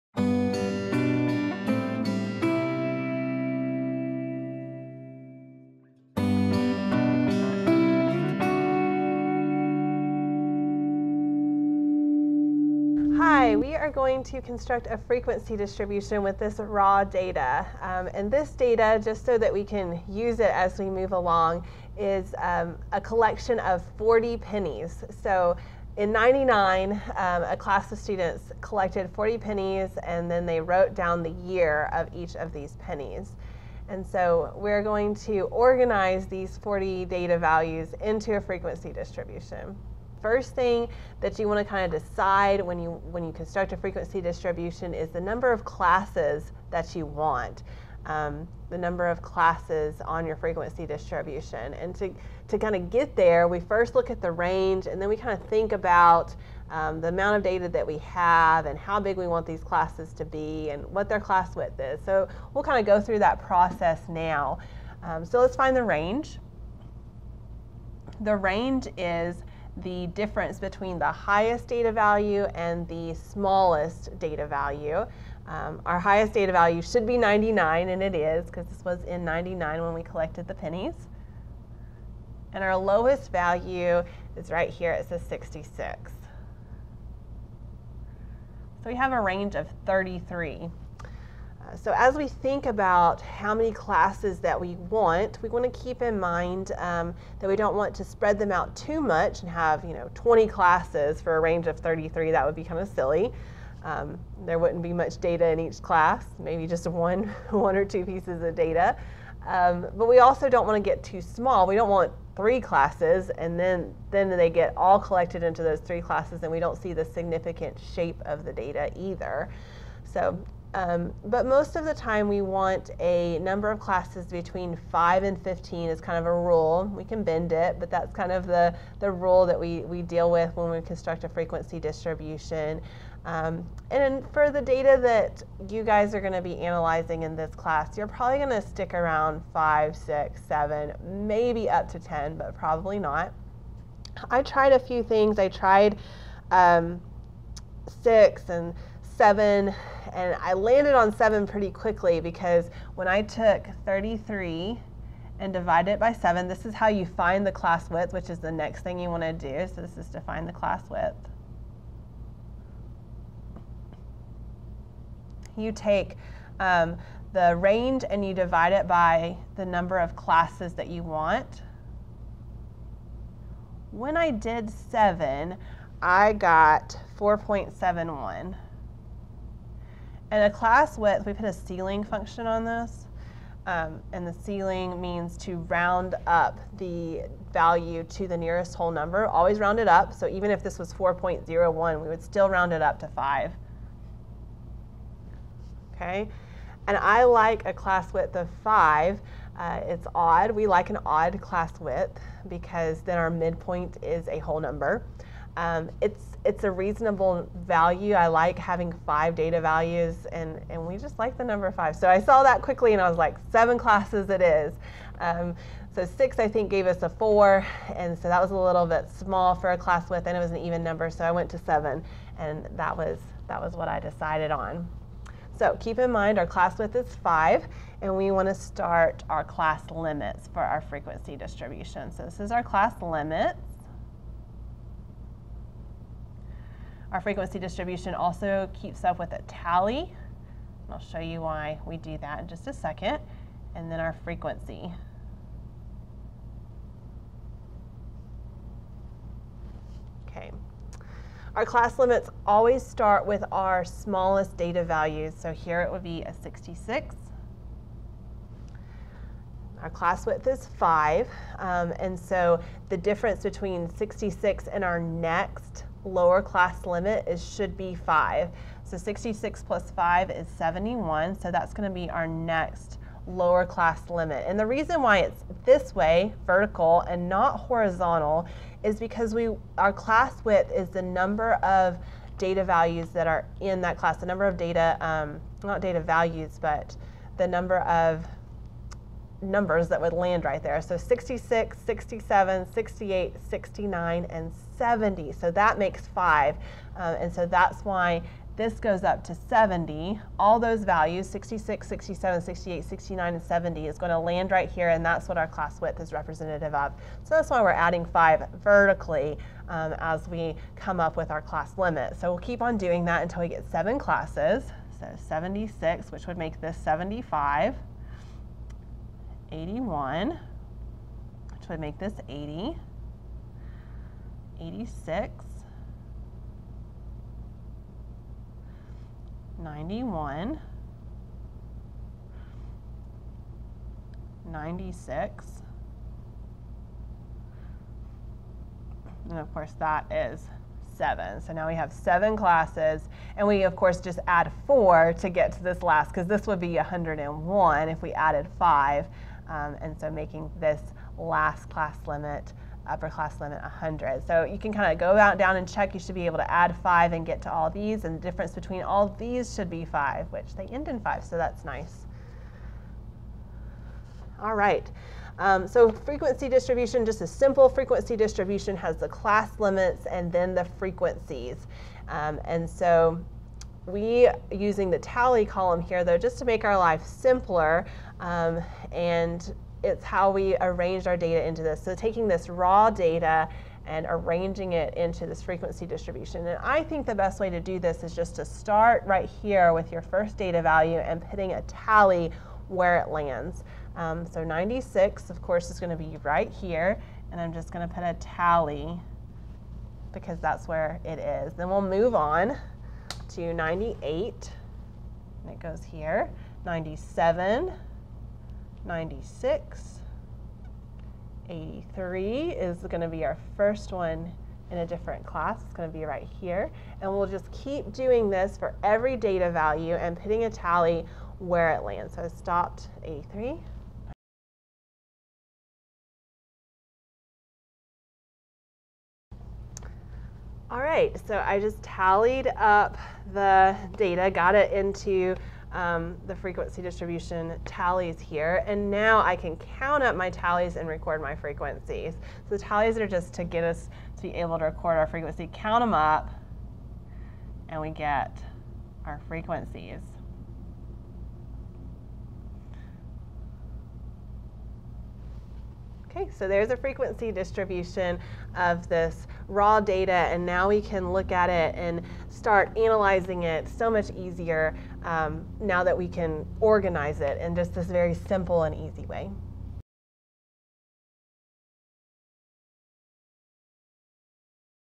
Constructing a Frequency Distribution
How to construct a basic, 3 column frequency distribution by hand from raw data.